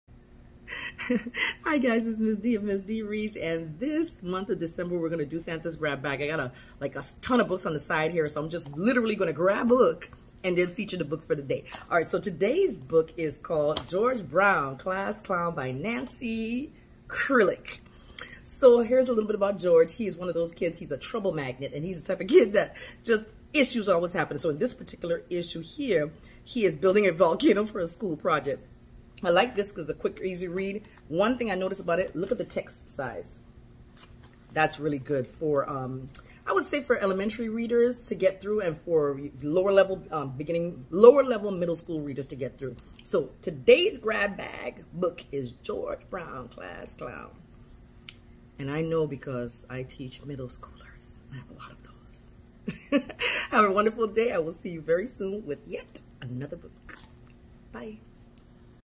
George Brown Class Clown - Ms. Dee Reads
The annual talent show at Shirley S. Sugarman Elementary School is in just a few weeks, and George signs up to be a stagehand. He is determined to do a good job because the new, improved George is responsible and helpful and glad to pitch in. So is comic mayhem coming? Oh yeah, absolutely! But leave it to Nancy Krulik to devise an unexpected way for disaster to strike and bring down the curtain.